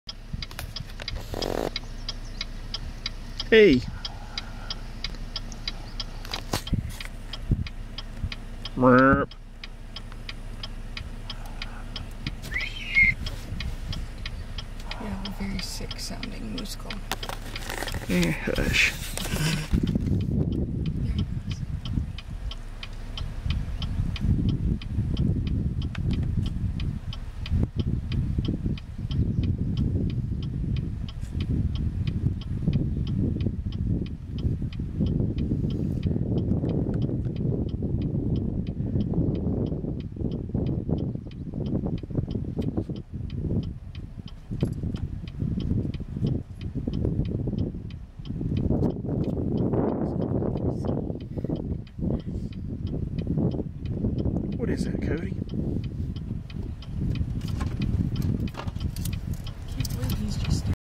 A Moose Near Logan, Utah
We saw this little guy today. Never saw Mom though.